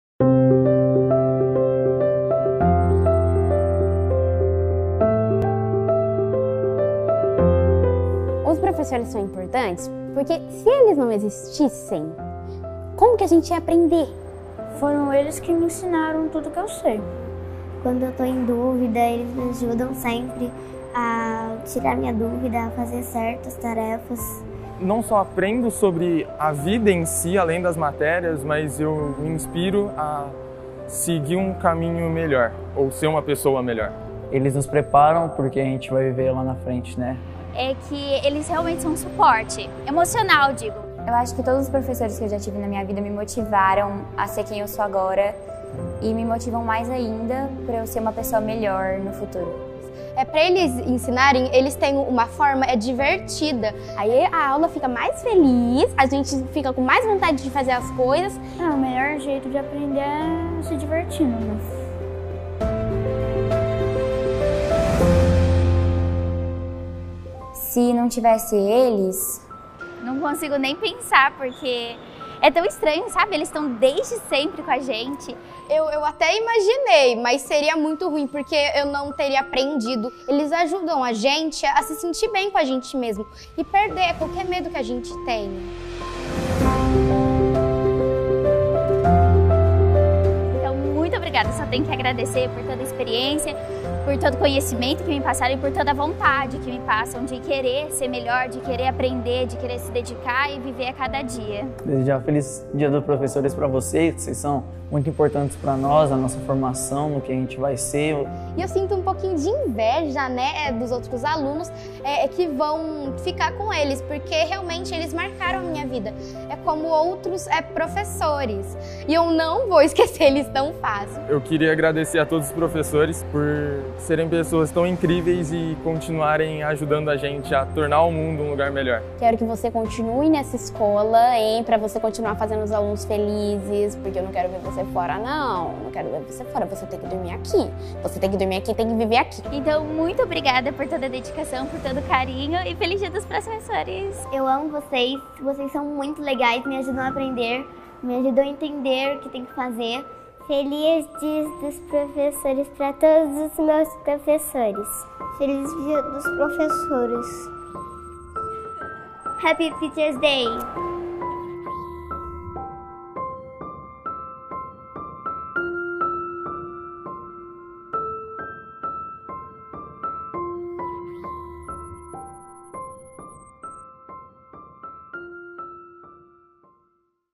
Como seria a sua vida sem os professores?
Uma homenagem da Conexia Educação para todos os educadores.